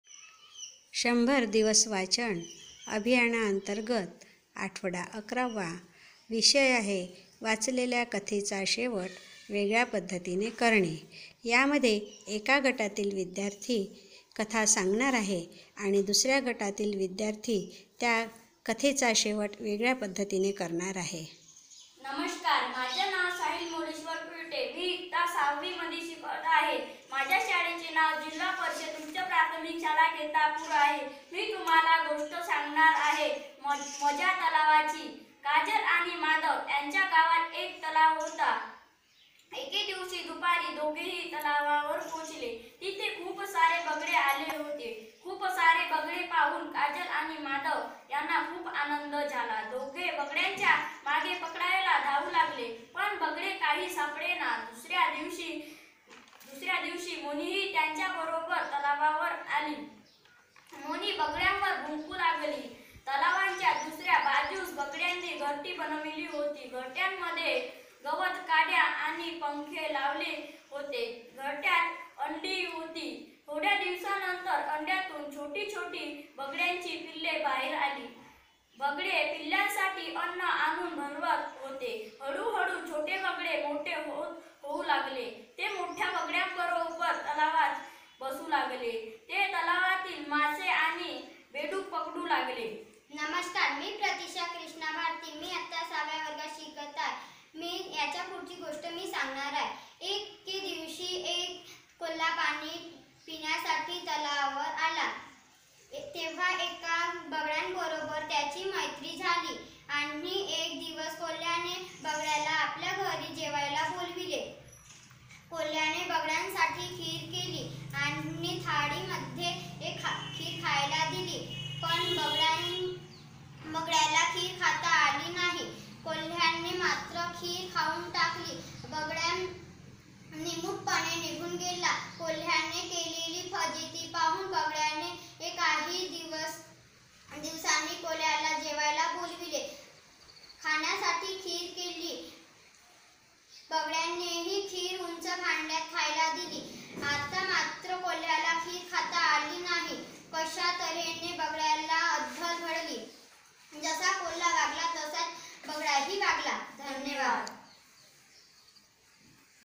वाचलेल्या कथेचा शेवट वेगळ्या पद्धतीने करणे "१०० दिवस वाचन अभियान आठवडा -११ वा"
उ.प्रा.शाळा खेतापूर, केंद्र डोंगरगाव, पं स कुही "१०० दिवस वाचन अभियान आठवडा -११ वा" अंतर्गत "वाचलेल्या कथेचा शेवट वेगळ्या पद्धतीने करणे" हा उपक्रम सादर करताना